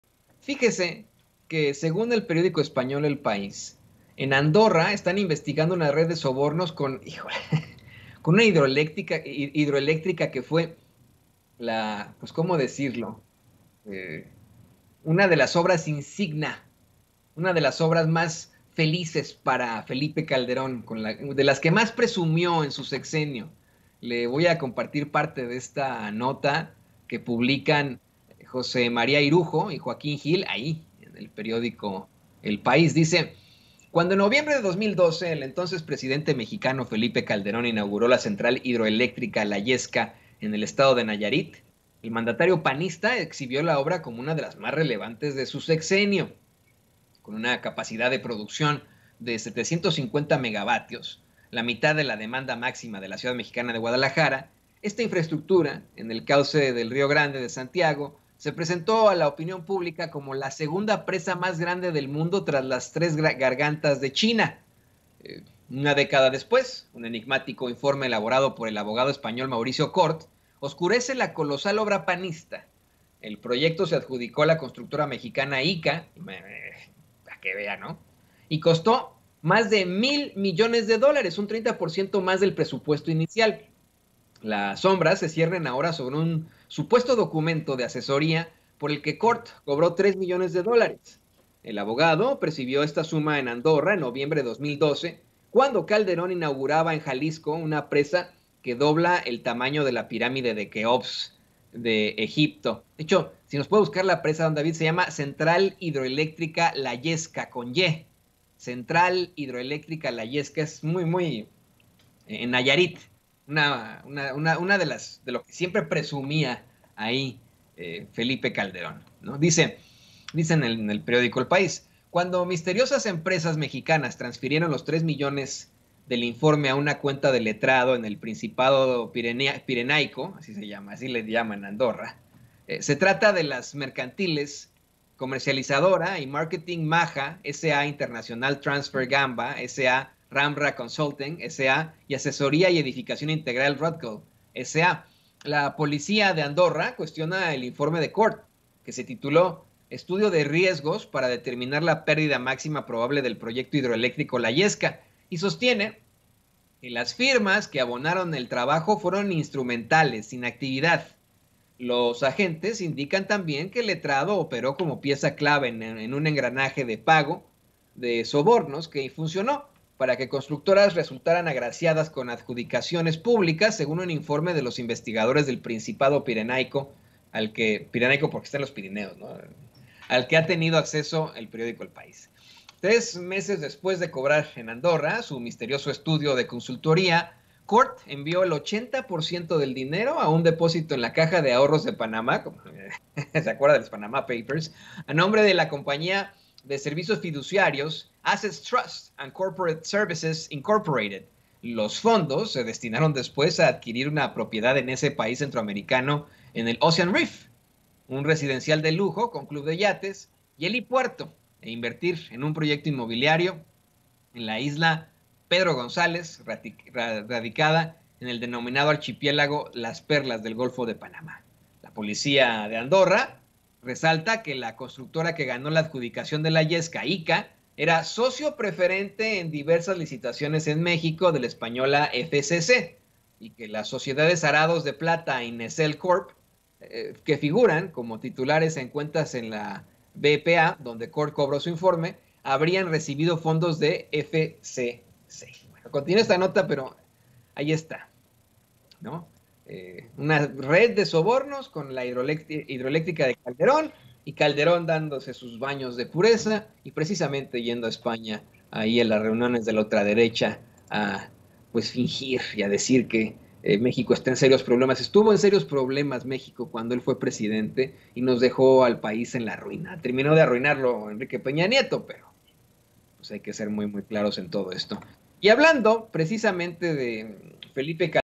EL PAÍS REVELA INVESTIGACIÓN EN ANDORRA POR RED DE SOBORNOS CON OBRA DE CALDERÓN
El periódico español El País reveló que se realiza una investigación en Andorra ante una presunta red de sobornos en una de las obras que más presumió el carnicero de Michoacán Felipe Calderón, la Central Hidroeléctrica La Yesca